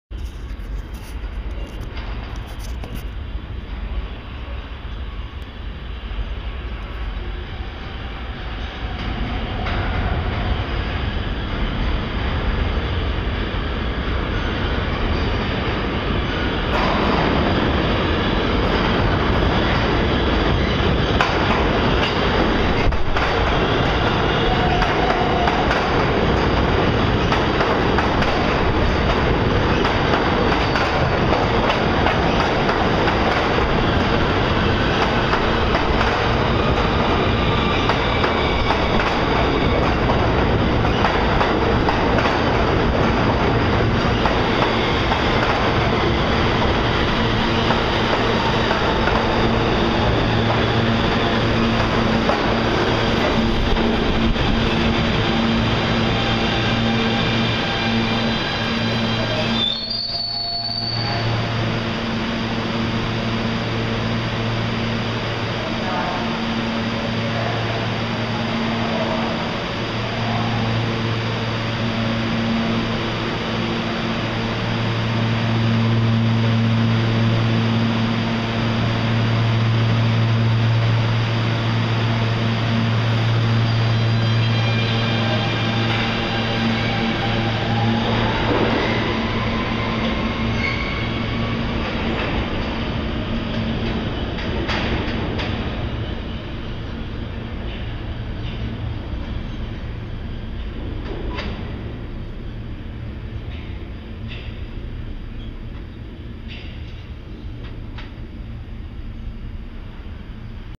North of Union Square
Train entering the station at 14th Street Union Square.